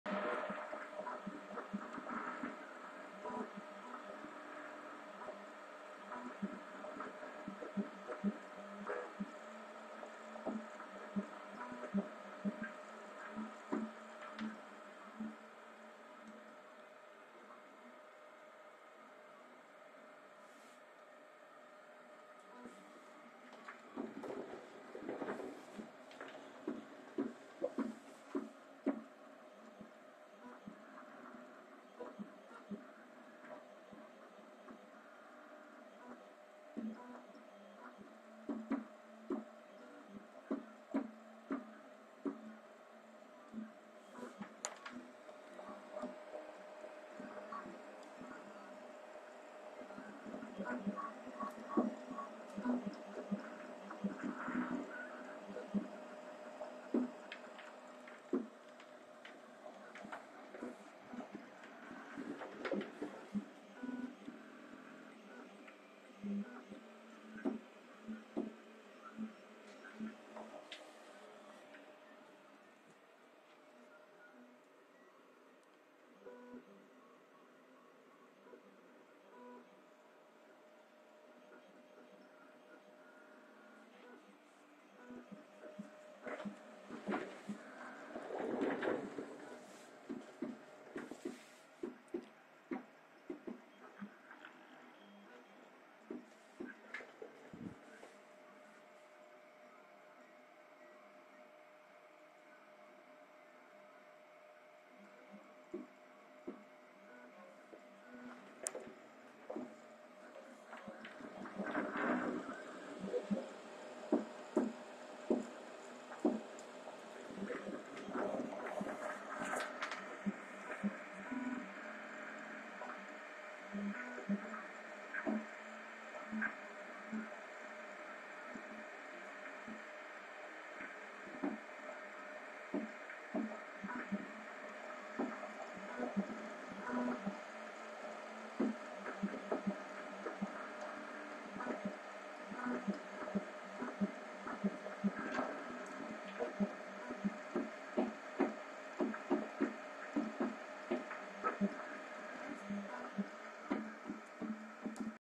3D Printer / Router EML Dimension5 ver4 (First 3D Print)
This is the very first print (of a whistle) for this machine...